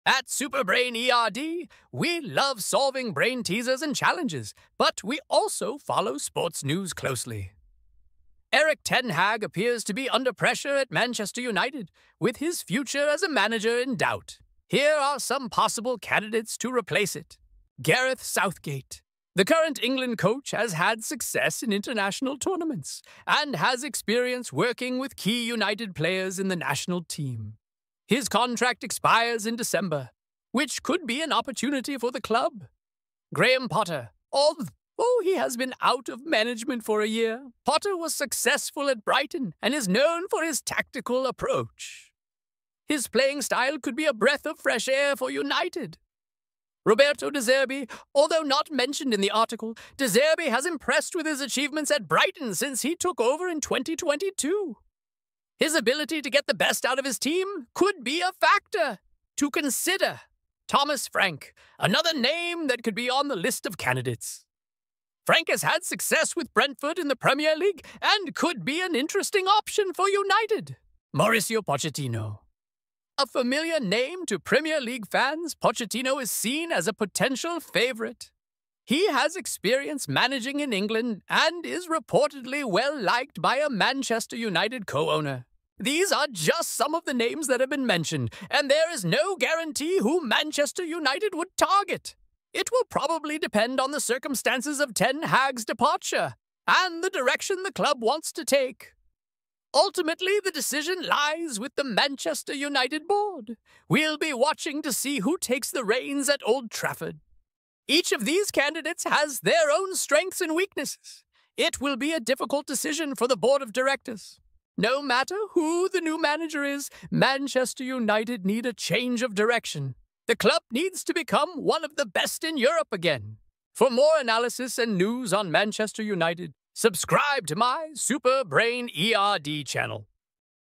TEN HAG OUT OF UNITED? 5 CANDIDATES TO REPLACE HIM! ⚽️
BOMBSHELL! Erik ten Hag's future at Manchester United hangs in the balance. In this video, we analyze 5 potential candidates to replace him on the Old Trafford bench.

Who do you think should be the new Red Devils manager?

Don't miss this video full of surprises and debate!